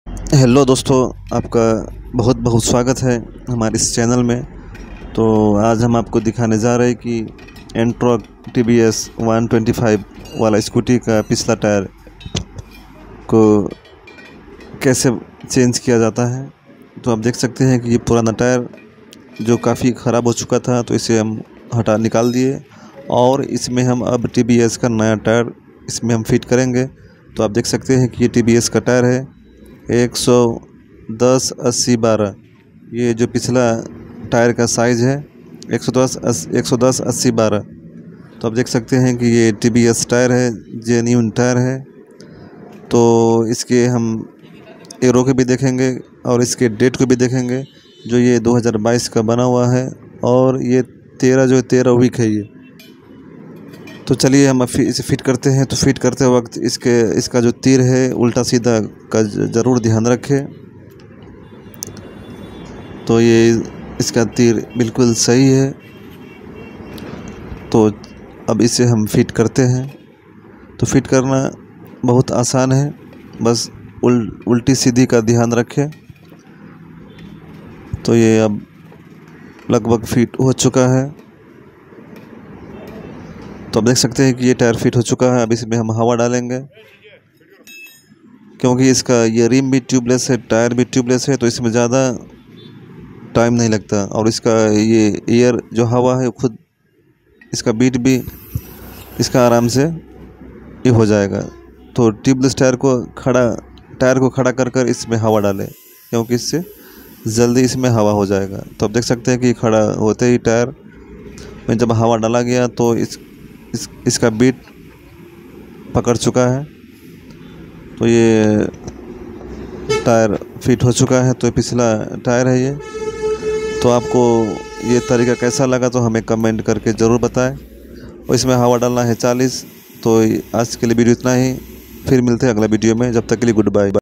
Tubeless Tire खोल्ना और फिट करना सिखे || How To Open And Fit Tubeless Tire, Easy Trick To Fit Tyre
Tubeless Tire खोल्ना सिखे || How To Open Tubeless Tire From Rim, Easy Trick To Open Marco Polo Tire